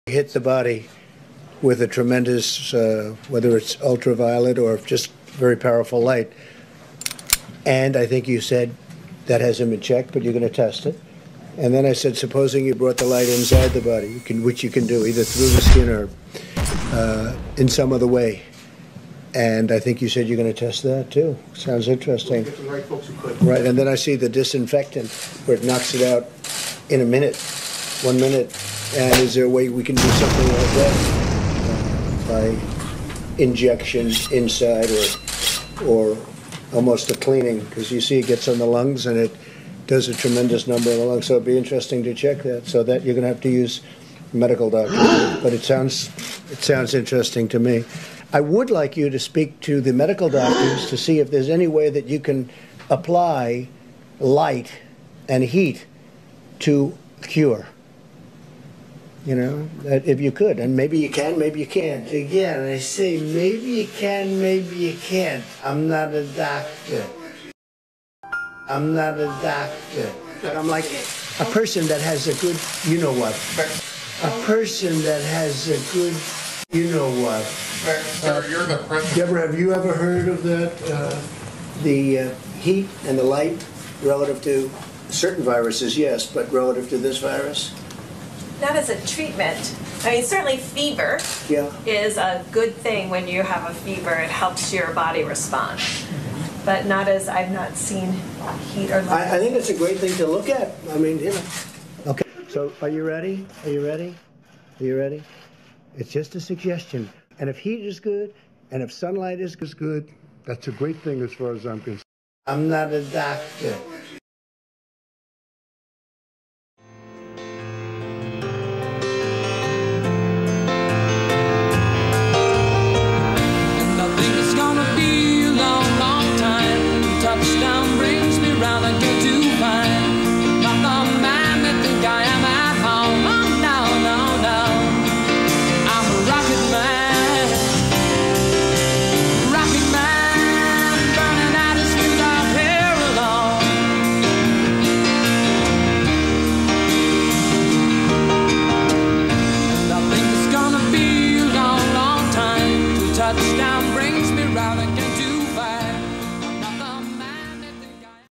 Trump impresses people with his unbelievable intelligence: coronavirus edition.
Covid-19 cures for dummies. Trump impresses the worlds top virus specialists with his understanding of heat and light. Bonus: Trumps touching tribute to Kim Jong Un.